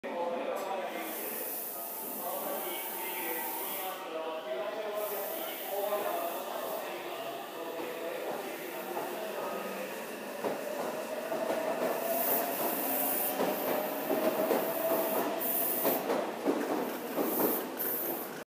【名鉄】2000系ミュースカイ岐阜行 金山発車
金山16:32発 ミュースカイ岐阜行 土休日37レ
中部国際空港16:07発 ミュースカイ岐阜行 岐阜17:04着